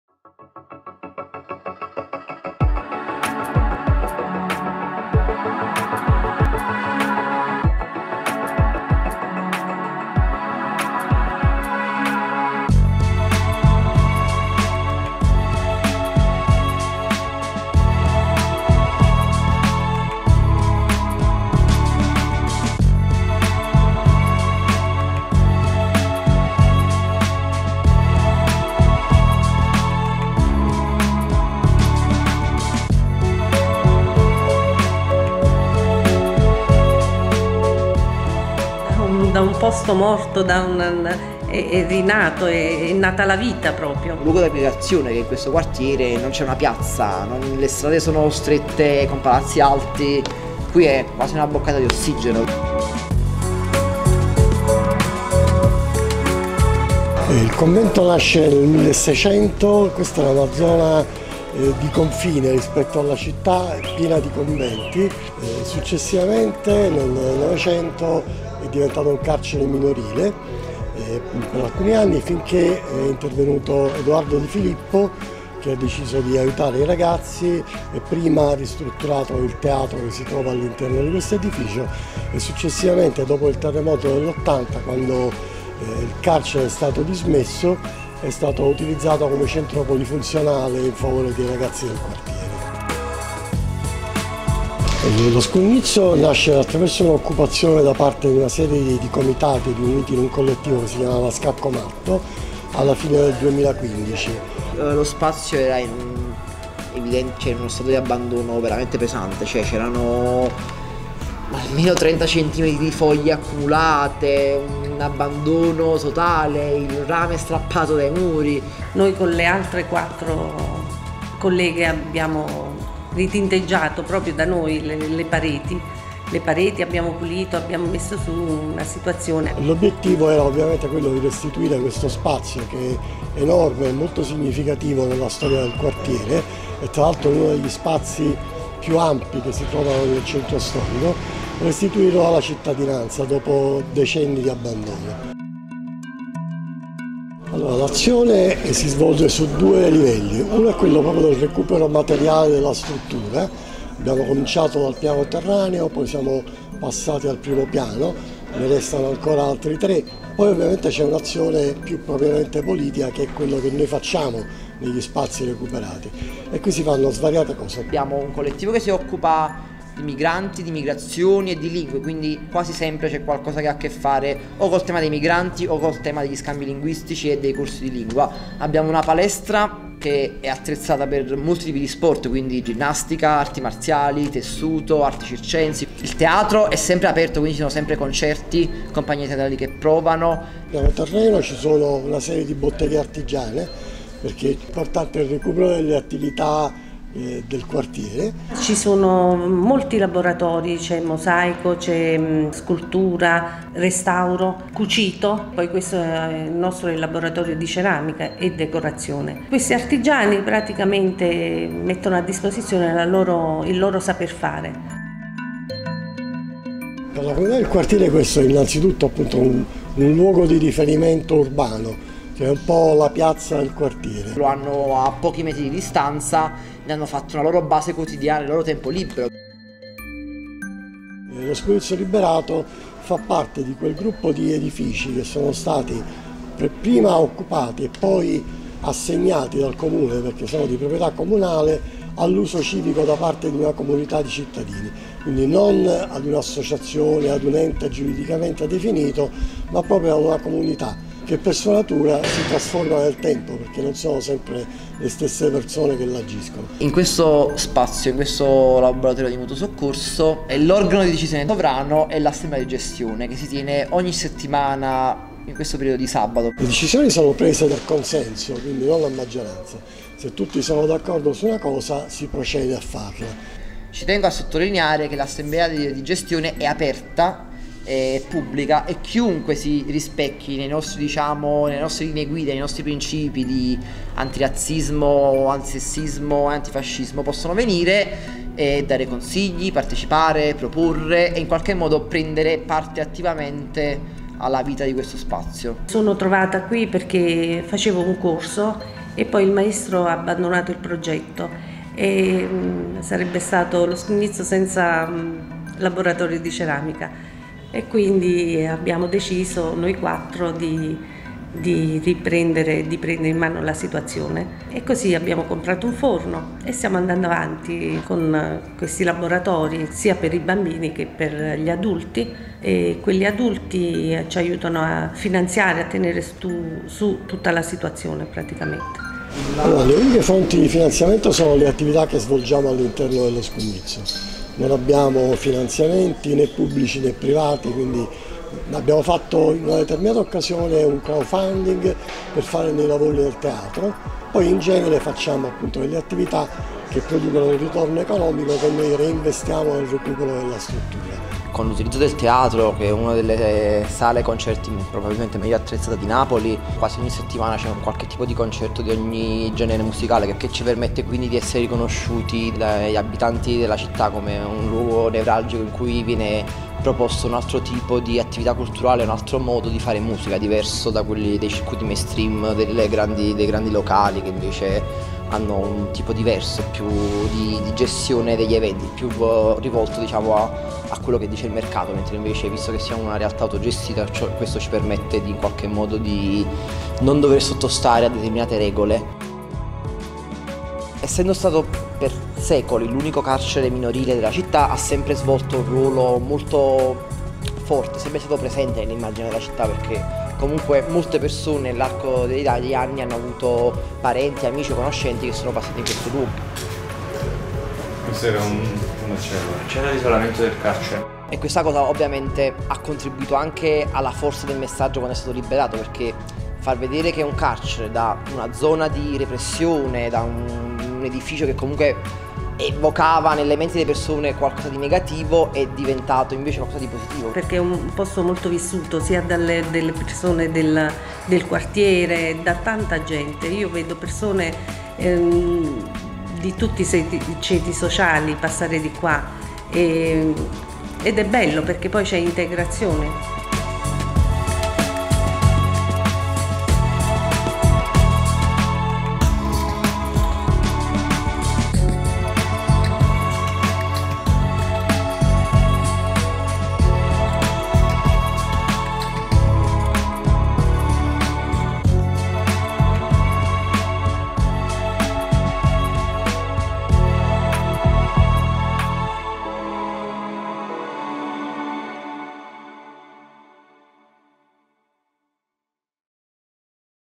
Convento delle Cappuccinelle in Naples
The former church complex Convento delle Cappuccinelle, situated in a very dense neighborhood of Naples historical center, was a juvenile jail until the end of the last century when it was definitively dismissed. On 2015 a local grass-roots group (“Scacco Matto” / Check-mate) occupied it, aiming to find a social purpose for the empty complex. The building, re-named “Scugnizzo liberato” (Freed street child) was partly self-restored by the occupants and now is hosting mutual activities (such as language courses, after-school, sports, dance, and theatre) and spaces for coworking, art and craft labs. Naples municipal government, the current owner of the complex, decided to support the occupant group and recognized – with a public resolution – the social value of the activities run in the complex, giving to the occupants the possibility to stay and to run the structure through self-management.

The Convento delle Cappuccinelle is the OpenHeritage Observatory Case. Visit the project's website if you want to learn more about other Observatory Cases: openheritage.eu/practices/